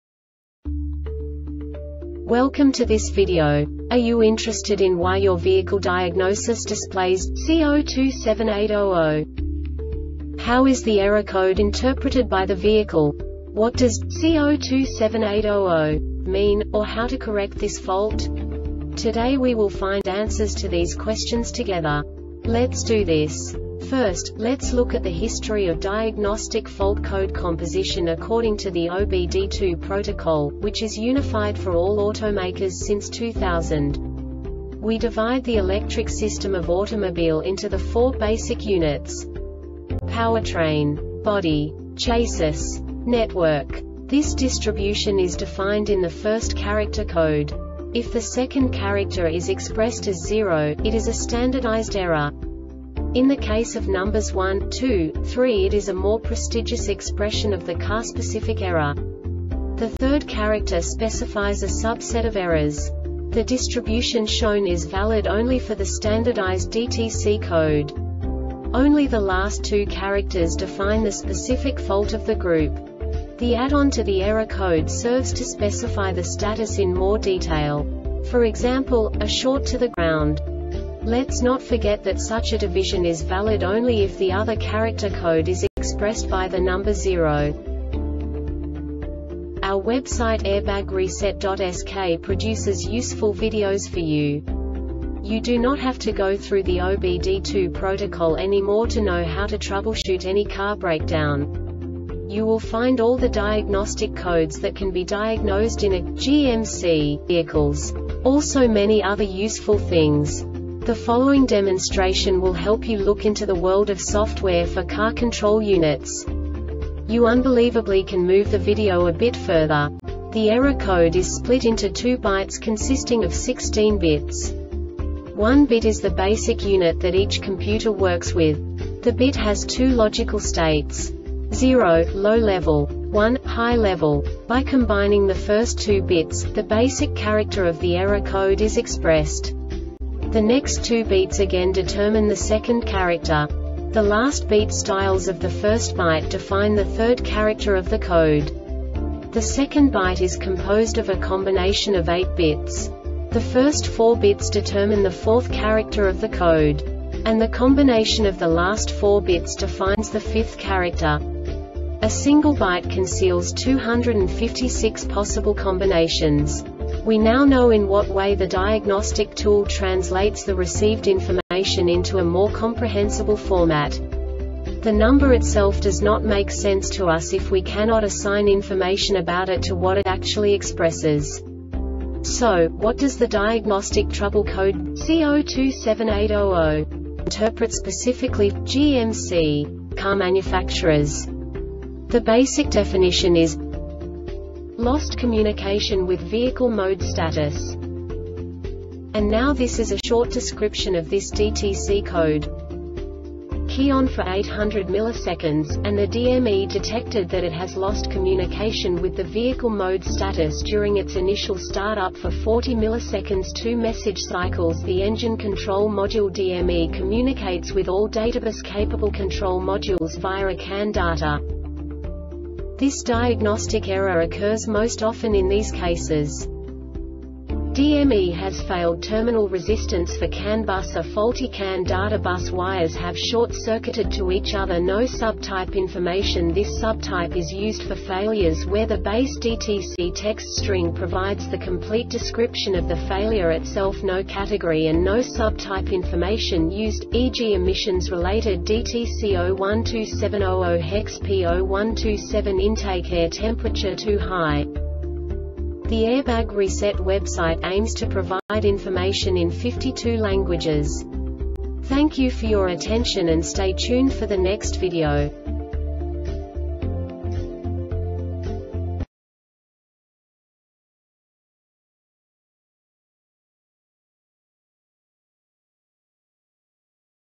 DTC GMC C0278-00 Short Explanation
Contents:
0:21 Basic DTC analysis according to OBD2 protocol standard.
1:48 Insight into programming
2:58 A diagnostically understandable description of the Diagnostic Trouble Code
3:05 Basic error definition

Make
GMC
Code
C0278 00
Definition
Lost Communication With Vehicle Mode Status
Description